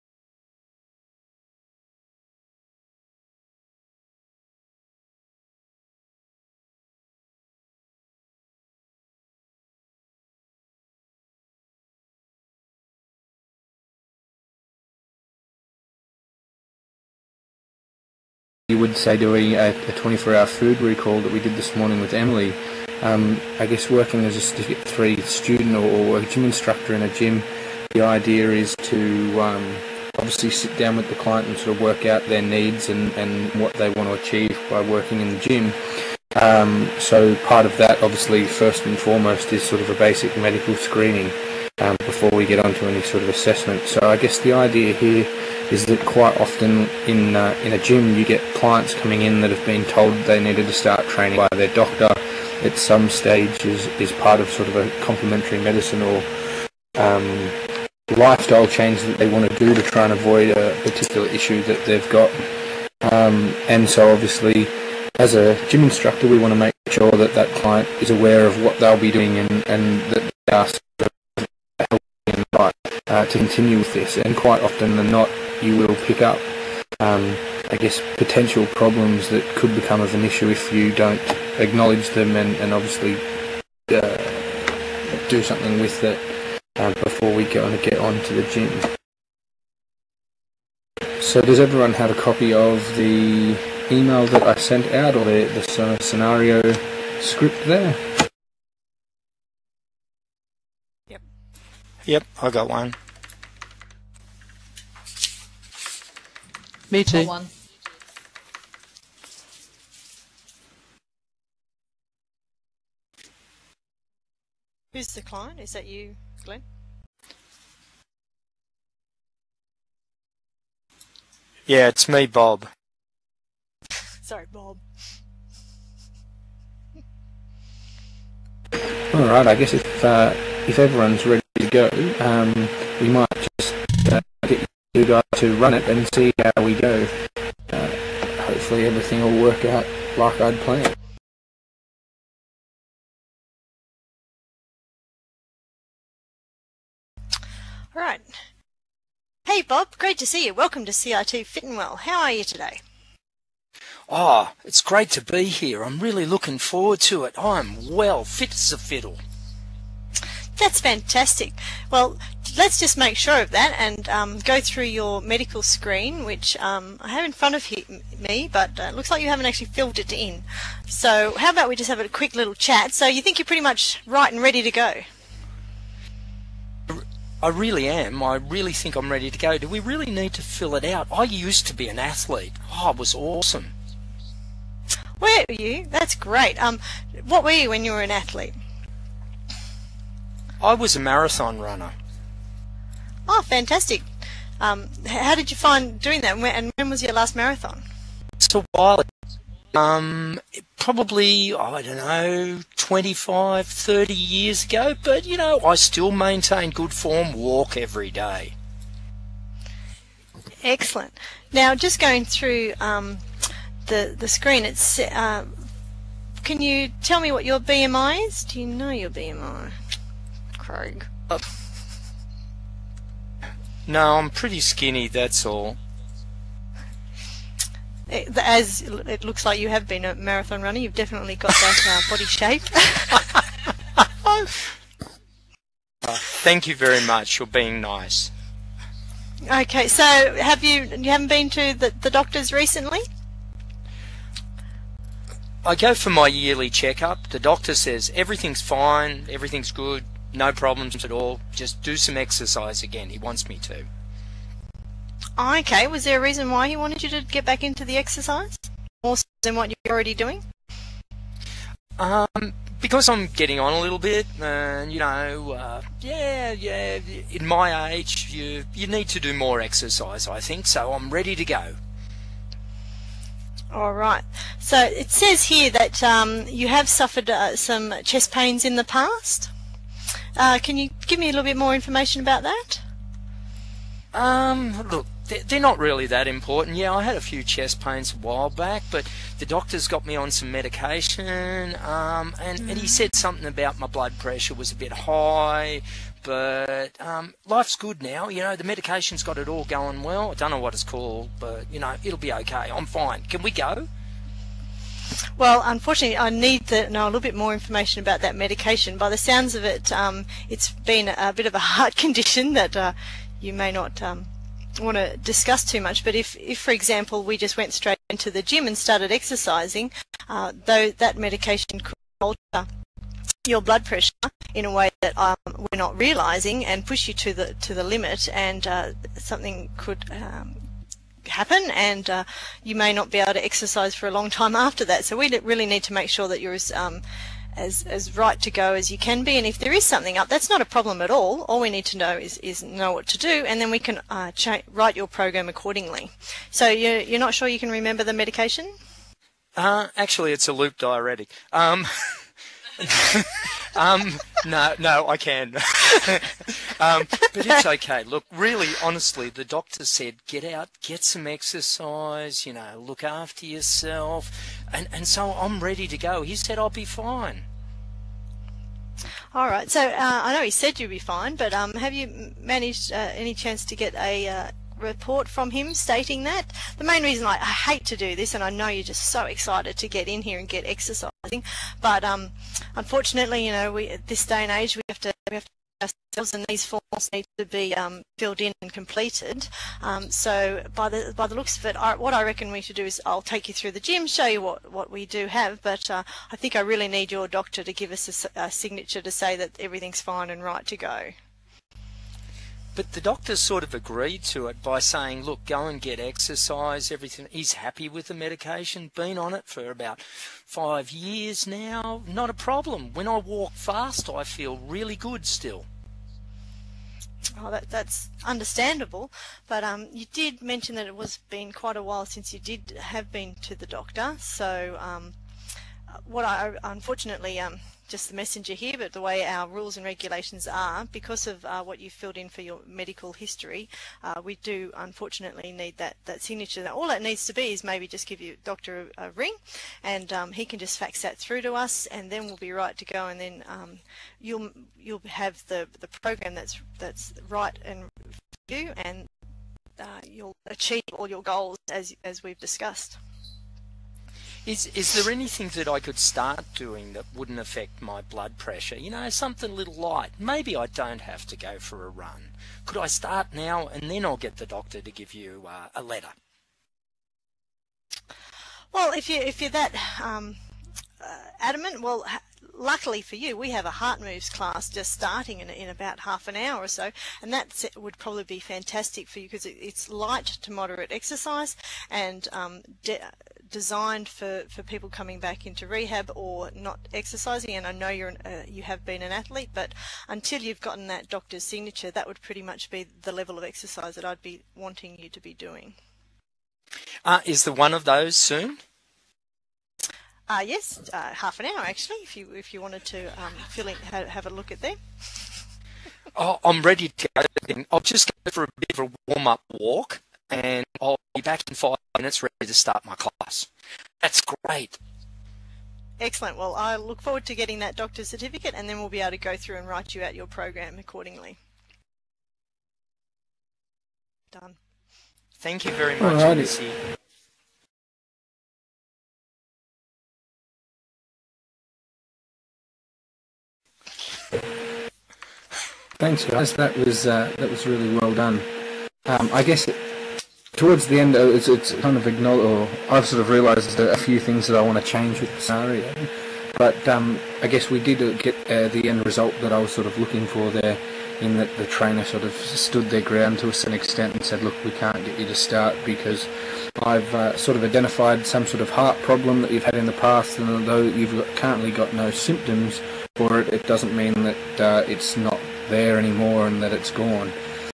CraigFinal.wmv
This video is part of the CIT VIrtual Worlds project. It invovles a conversation about screening for a fitness assessment